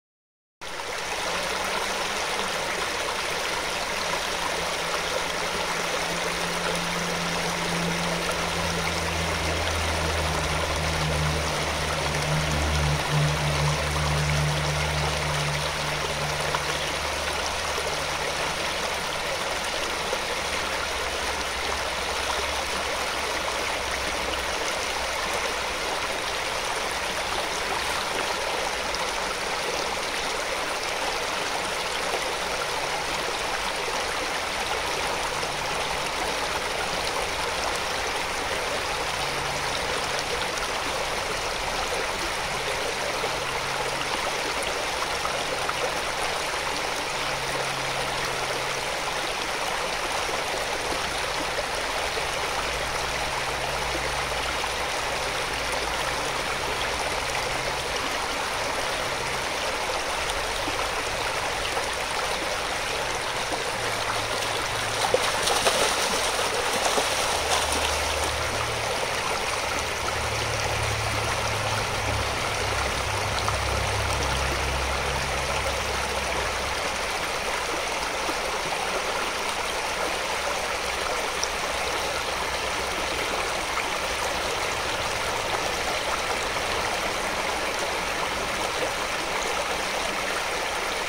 Find A Way And Keep Moving Forward (Wild Salmon Nature Scene Inspirational Short)
A wild salmon finds a way to level up and move forward on the path to destiny as it swims upstream to spawn. Nature Scene Inspirational Meditation Short.

Snohomish County, Washington, USA.